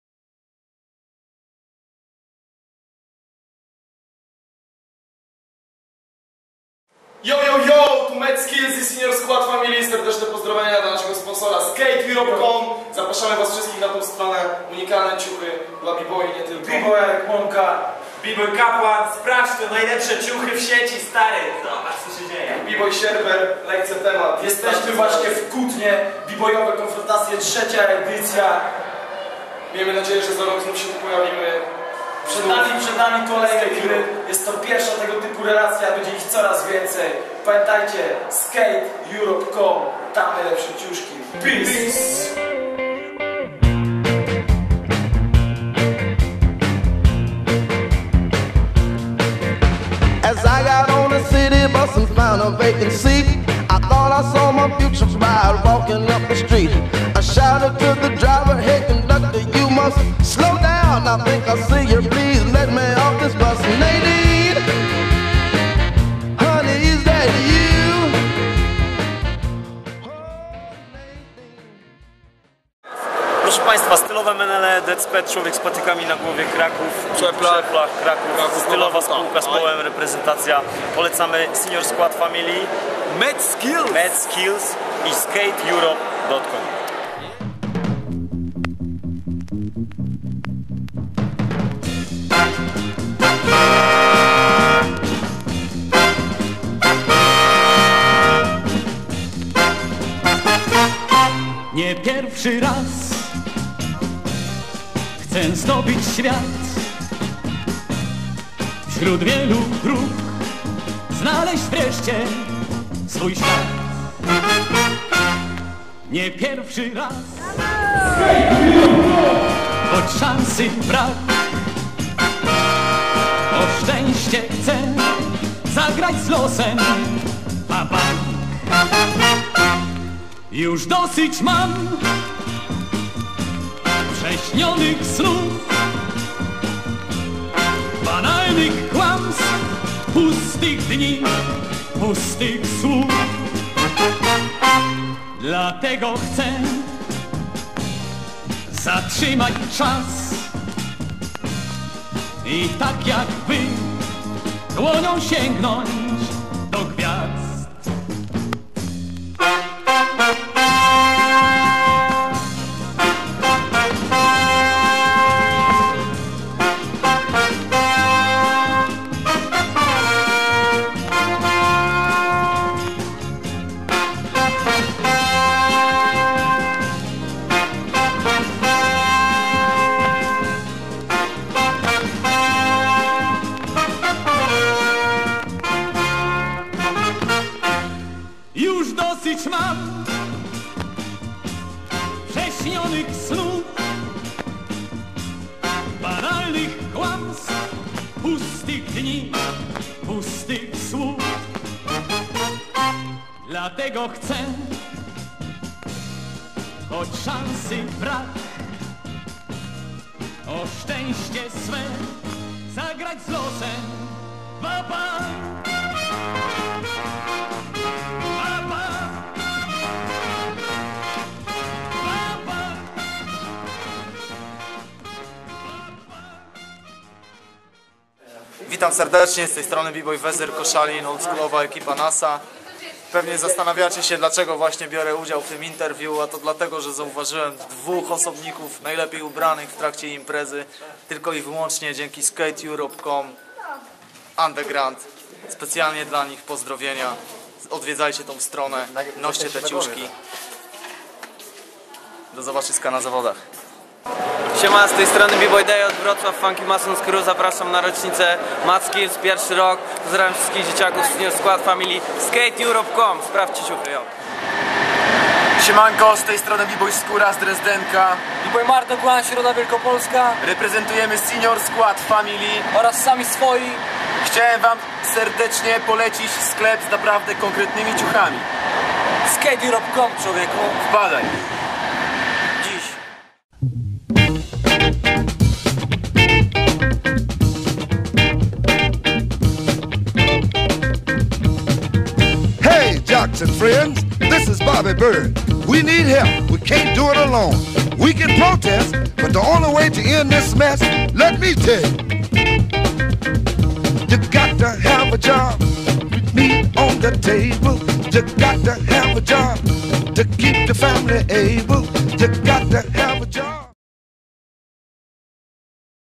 B boyowe Konfrontacje 3 - Kutno 2011 ( Skate-europe.com)
Krótki trailer z wyjazdu do Kutna na "B boyowe konfrontacje 3"
Mad Skillz & Sinior Skład Family